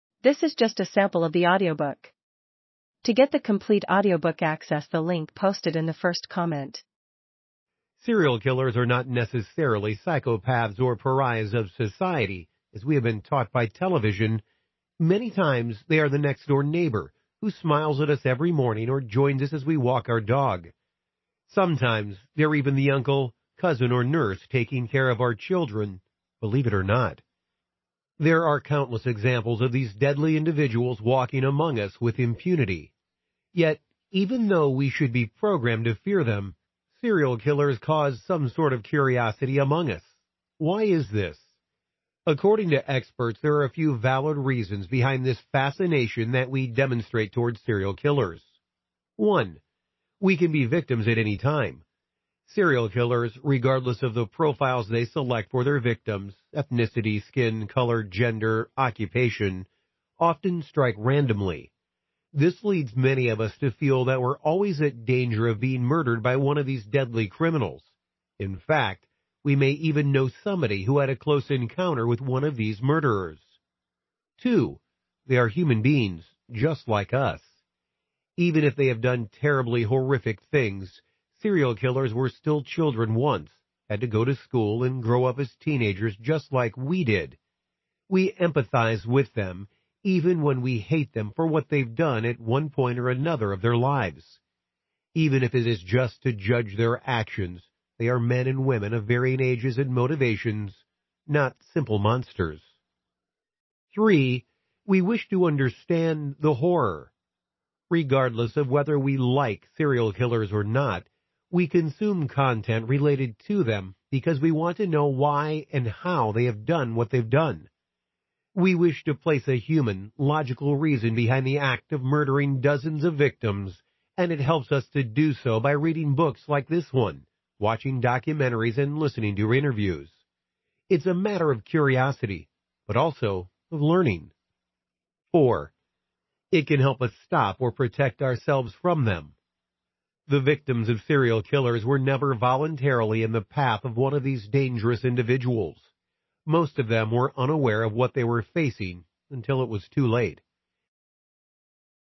The Big Book of Serial Killers Volume 2: Another 150 Serial Killer Files of... | Audiobook Sample
The Big Book of Serial Killers Volume 2: Another 150 Serial Killer Files of the World's Worst Murderers: An Encyclopedia of Serial Killers

Our first volume caused such an impact that we’ve decided to bring you the long-awaited Volume 2 of the most comprehensive Serial Killer encyclopedia ever published!Murderers or monsters, normal people turned bad or people born with the desire to kill; it doesn’t matter where they come from, serial killers strike dread into our hearts with a single mention of their names. Hunting in broad daylight or stalking from the shadows, we are their prey and their hunt is never done until they are caught or killed.With a worrying number of them living in our communities, working alongside us at our places of employment and sharing the same spaces where we spend time with our families, serial killers are typically just another neighbor that we barely think about.